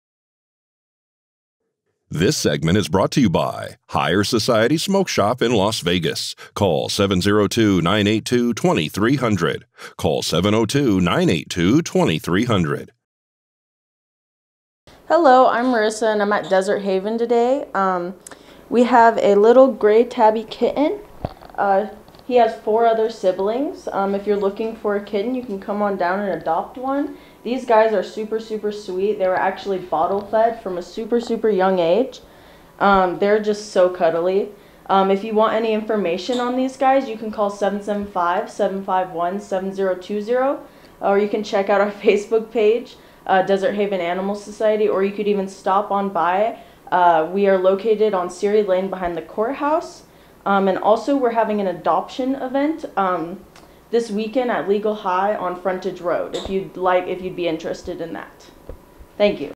06/10/2022 Save A Pet - Cute Kittens
Let's head on over to Desert Haven Animal Society where we catch up with Marissa Mont-Ros and a whole lot of cute kittens.